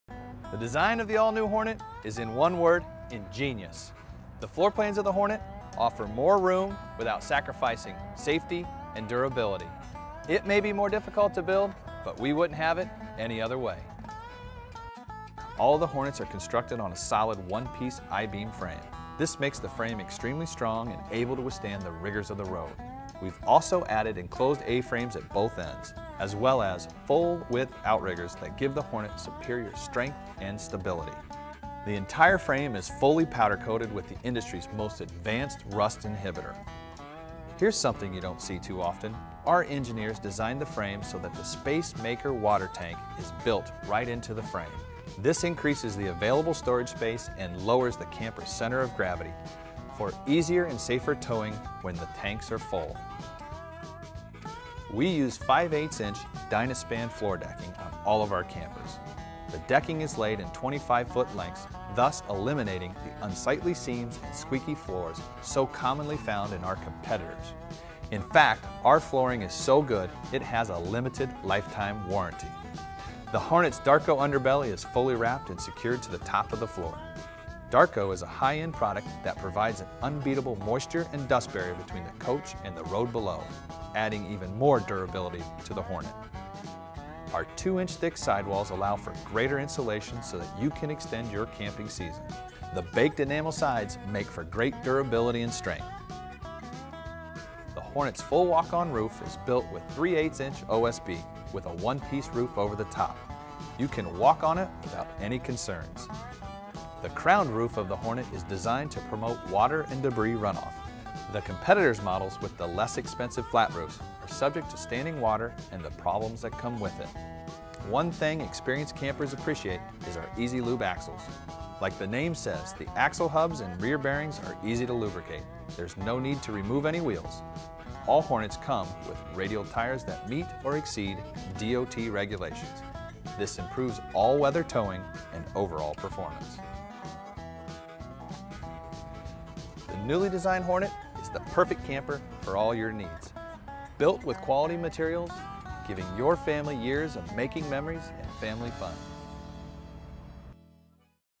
Construction - Keystone Hornet
Enjoy a detailed tour of the new Keystone Hornet and see how the design and hardware work together to create the fastest growing popular priced travel trailer on the market.  If you like to see how it's built BEFORE you buy it, this video is for you.